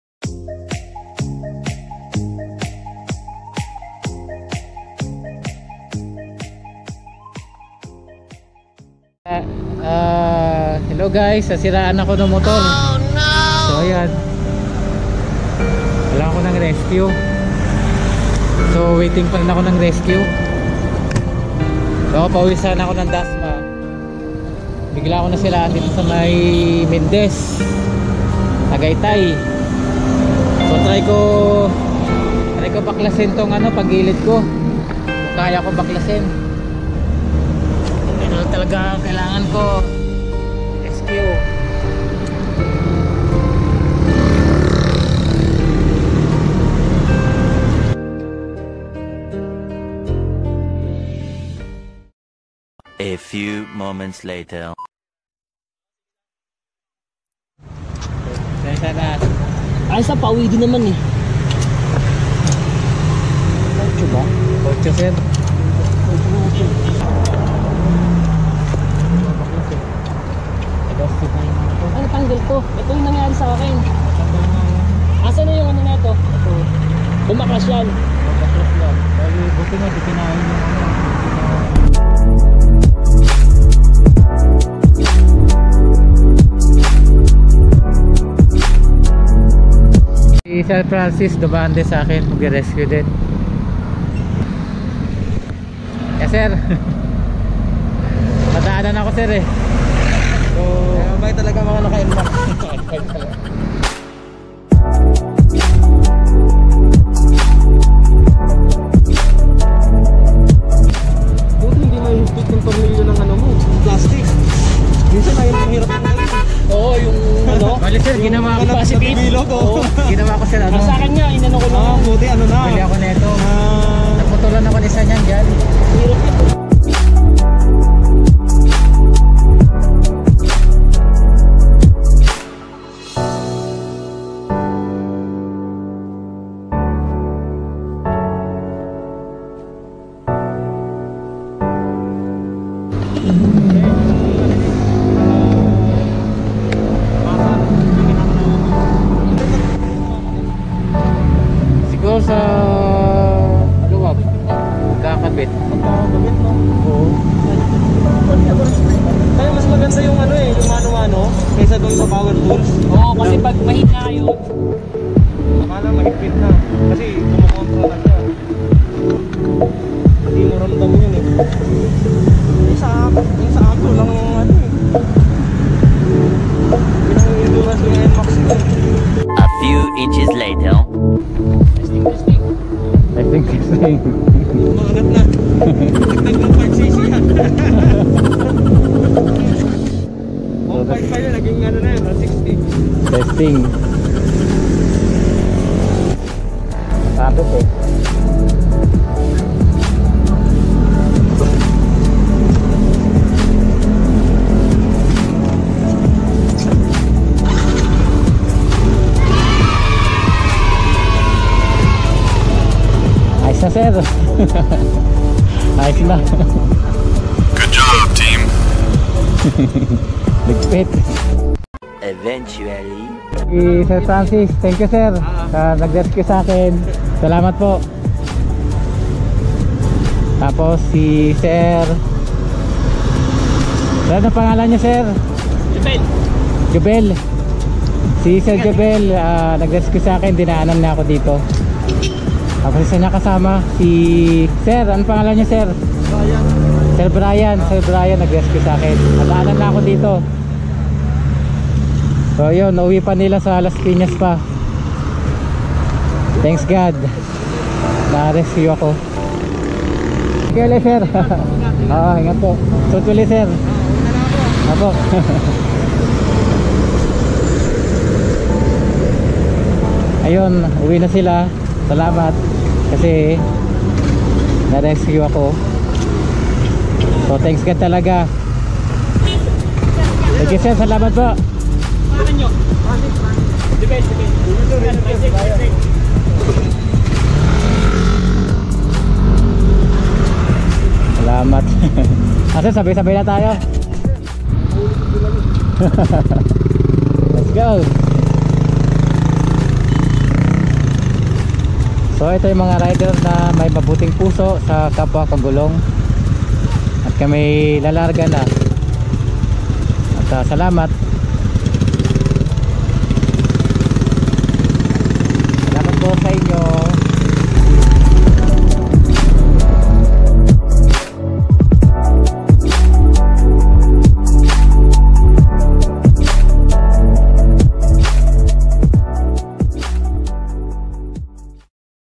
Nasiraan ako sa Daan/Salamat sa mga nagrescue
At nang masiraan ako sa daan slamat sa mga nagrescue sa dalawang rider na hnde  nagdalawang isip na hintoan ako sa tabing kalsada at tinulungan ako at kay sir francis na on da way for rescue din...slamat salute sa inyong lahat godbless po